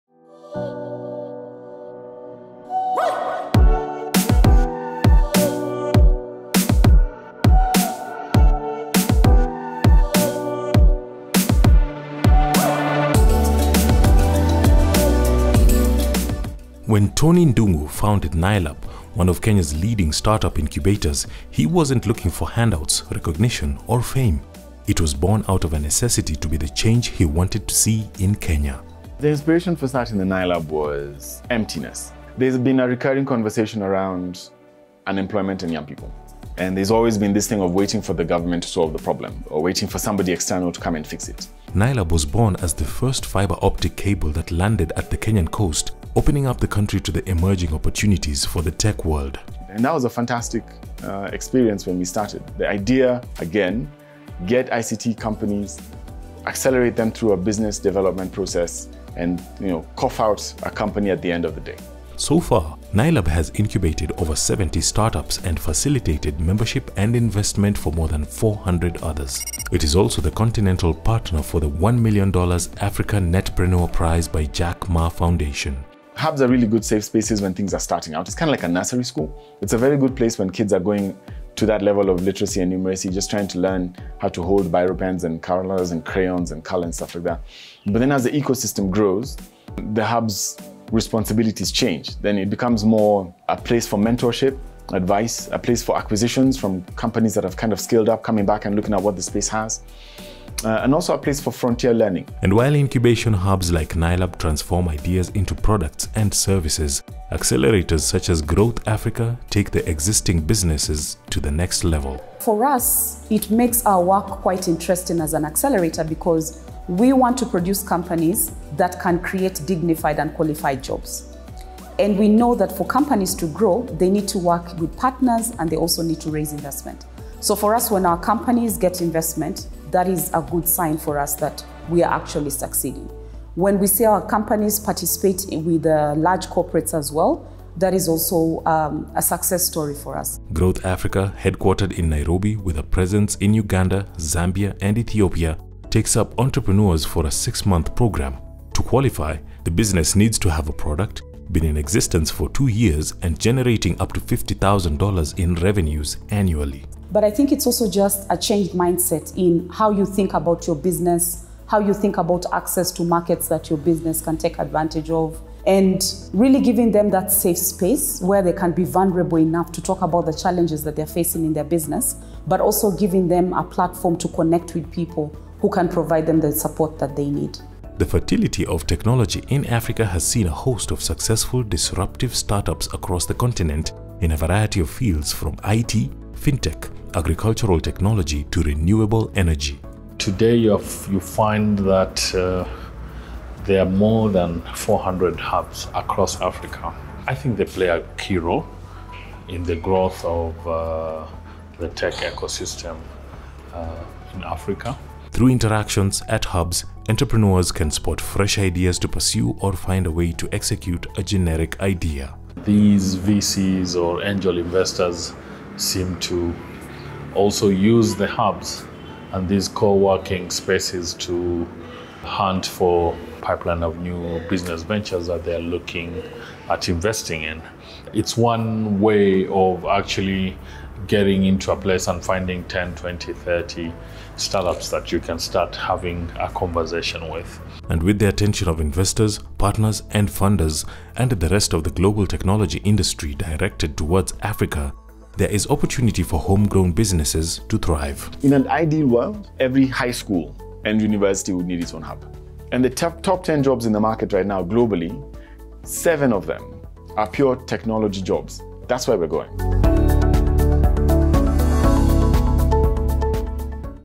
KCB Venture: The Art of Incubating Start Ups-The Nailab Story
Nailab was started to fill a gap that existed in the technology space. This is the story of an incubator that has changed the lives of many start-ups in Kenya.